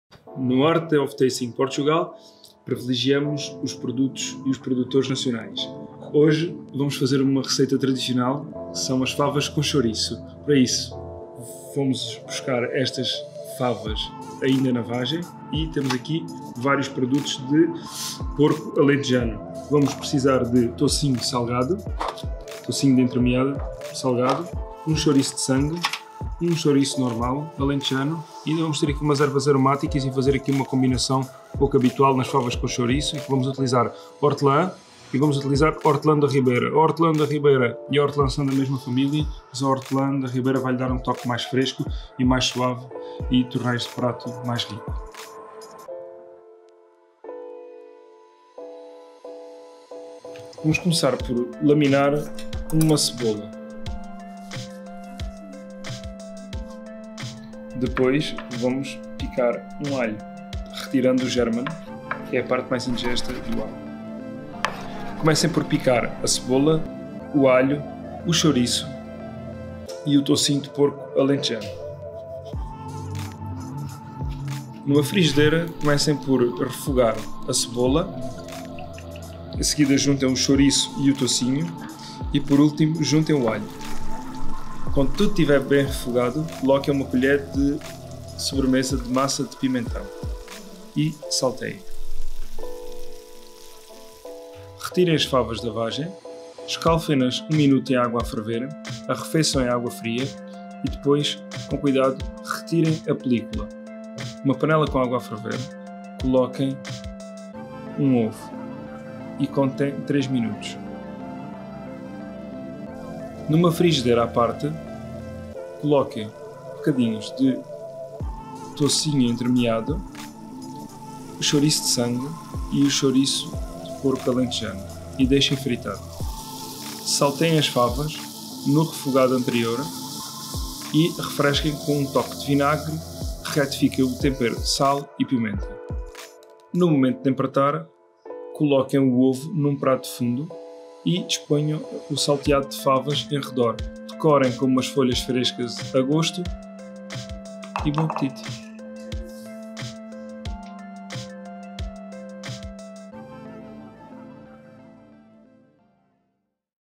Favas com chouriço pela mão do chef Carlos Afonso
E se...
O Chef Carlos Afonso do @restauranteofrade partilhasse consigo uma deliciosa receita de favas?
Carregue no play e bom apetite!